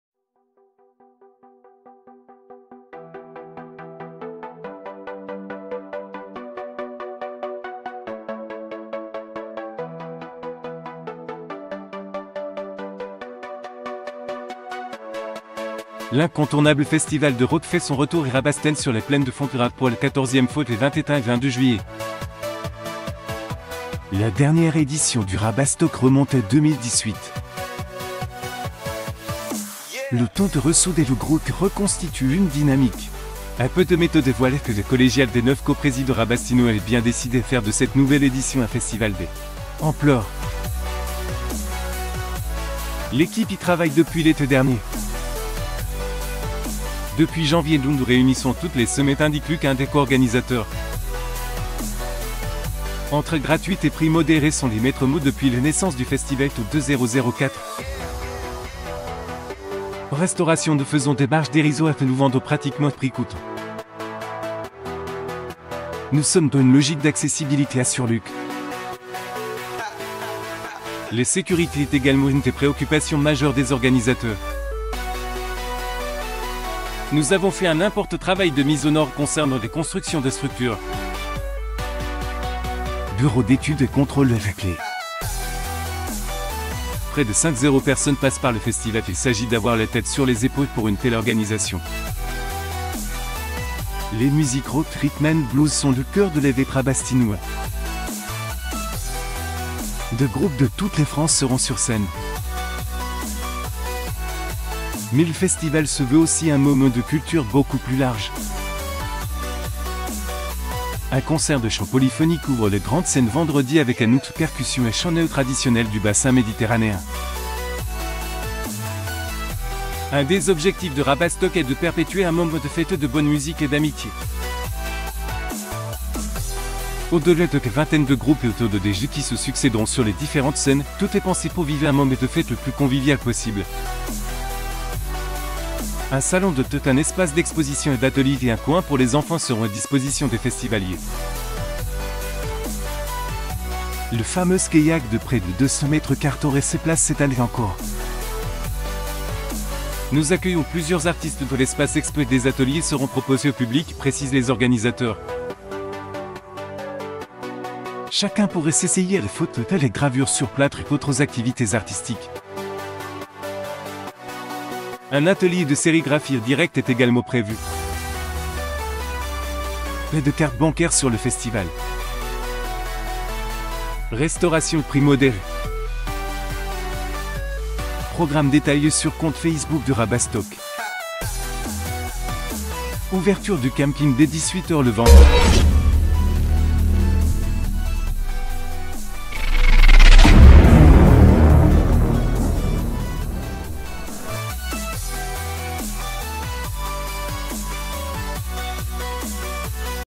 Tarn: Rabastock est de retour à Rabastens
,cuisine tarnaise

Tarn: Rabastock est de retour à Rabastens Thank BUY L’incontournable festival de rock fait son retour à Rabastens sur la plaine de Fontgrave pour la 14e fois les 21 et 22 juillet. La dernière édition du Rabastock remonte à 2018. Le temps de ressouder le groupe, reconstituer une dynamique, un peu de méthode et voilà que la collégiale des 9 coprésidents Rabastinois est bien décidée à faire de cette nouvelle édition un festival d’ampleur. « L’équipe y travaille depuis l’été dernier. Depuis janvier nous nous réunissons toutes les semaines » indique Luc un des co-organisateurs. Entrée gratuite et prix modérés sont les maîtres mots depuis la naissance du festival, en 2004. « En restauration nous faisons des marges dérisoires, nous vendons pratiquement à prix coûtant. Nous sommes dans une logique d’accessibilité » assure Luc. La sécurité est également une des préoccupations majeures des organisateurs. « Nous avons fait un important travail de mise aux normes concerna